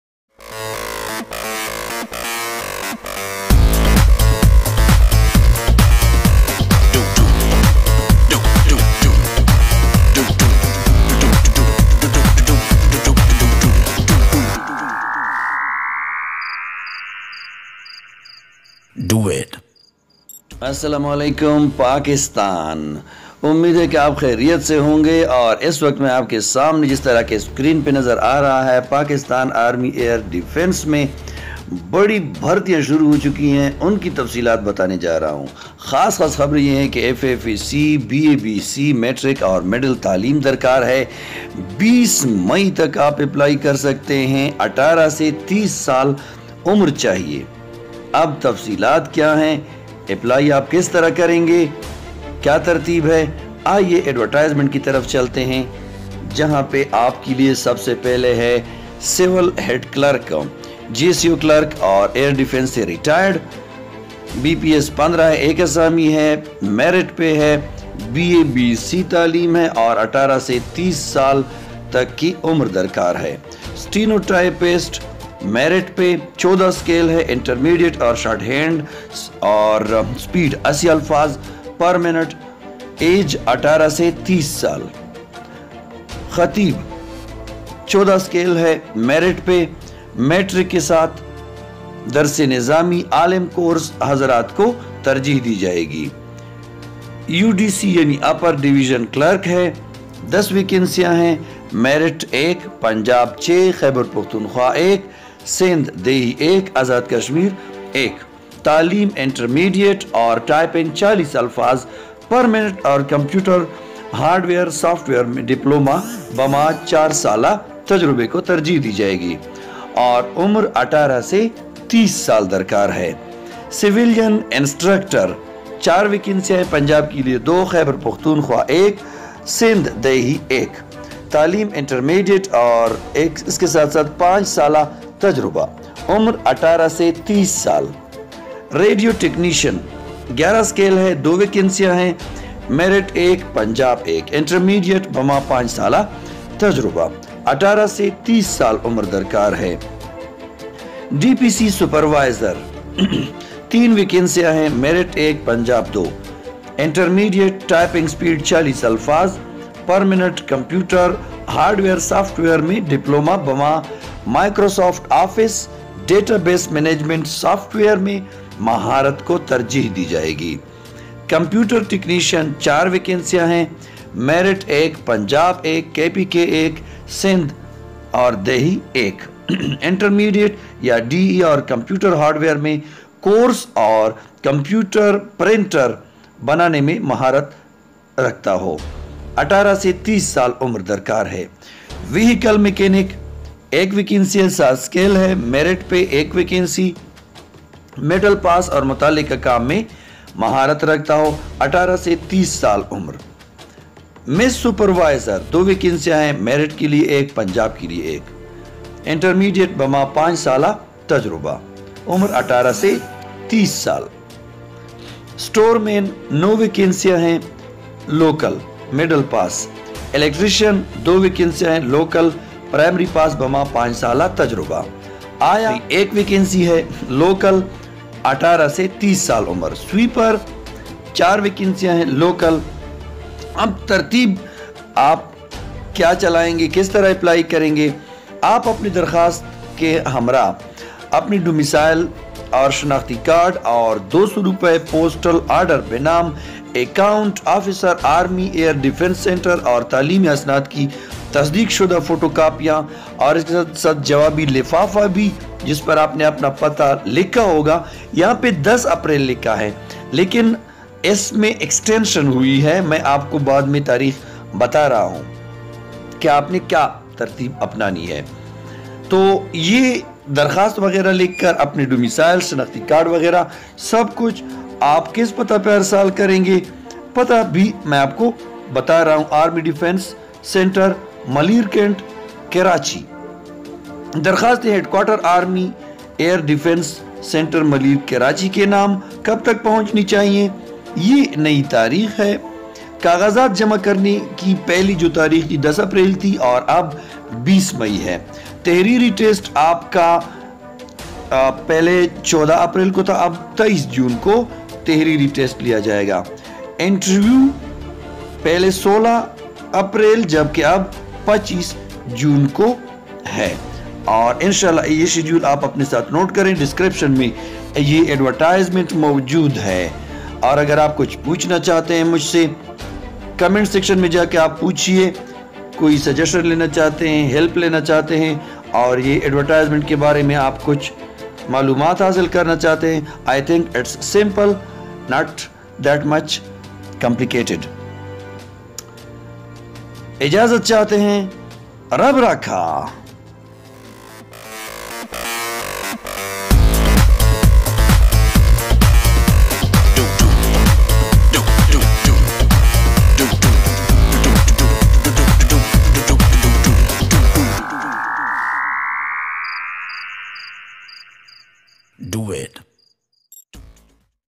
Join Pak Army Air Defence for Middle, Metric, Intermediate, BA, BSc.Apply by 20th May.
Join Pak Army Air Defence for Middle, Metric, Intermediate, BA, BSc.
Apply by 20th May.